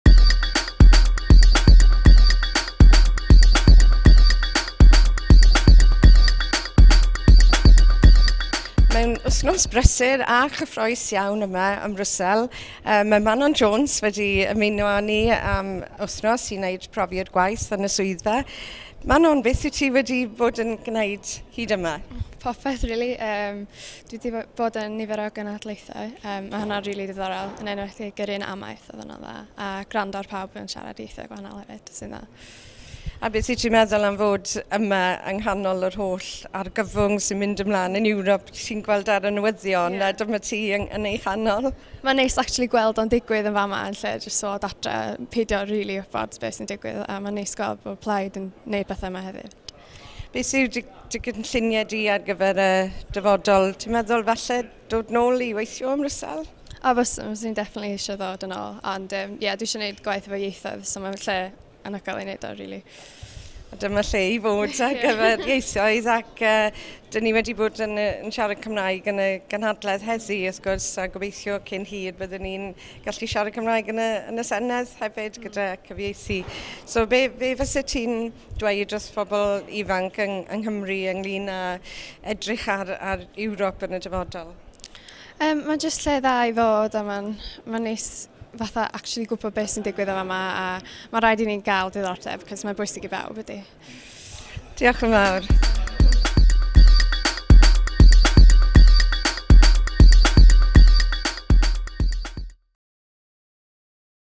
Jill Evans a Manon Haf Jones
Jill Evans yn siarad â Manon Haf Jones yn y gynhadledd EFA.